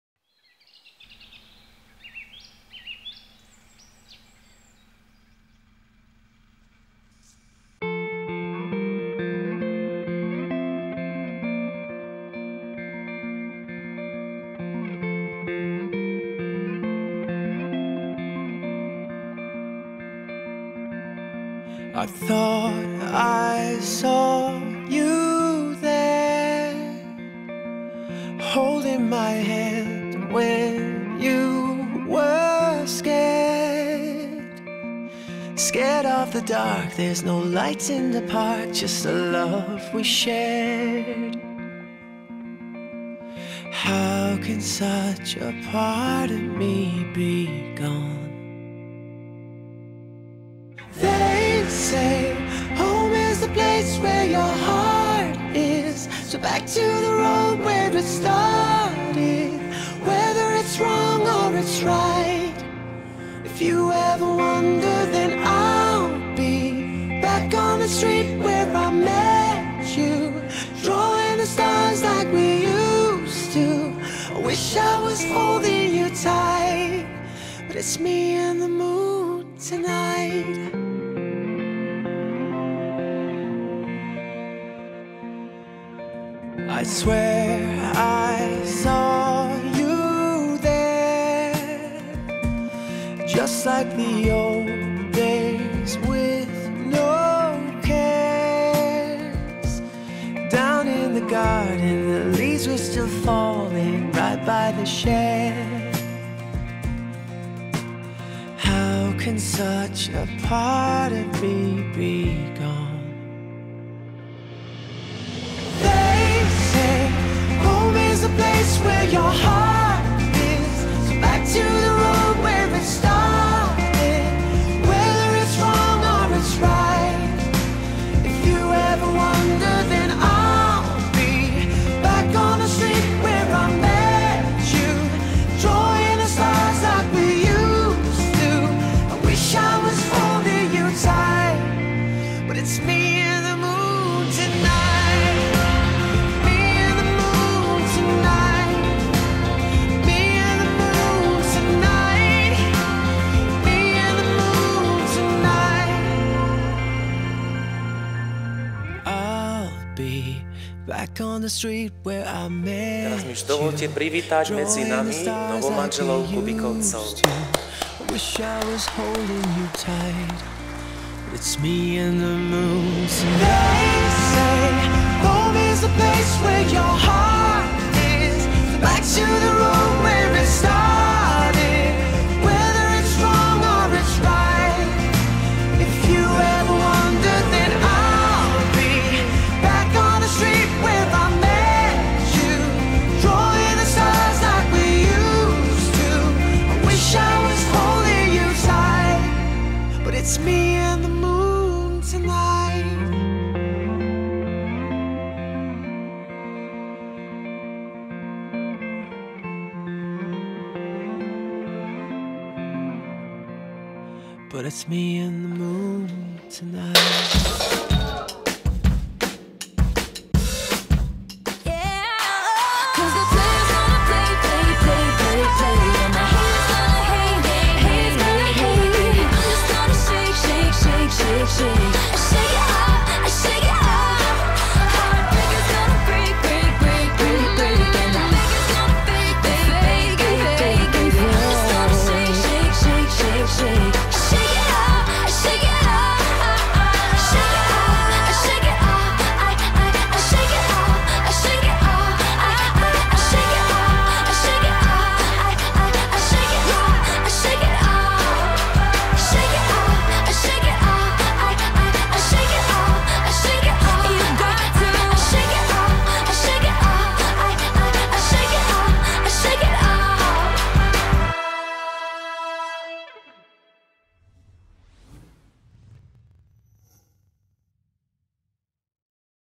Svadba Veronika & Michal
Svadba sa konala v Gbeľanoch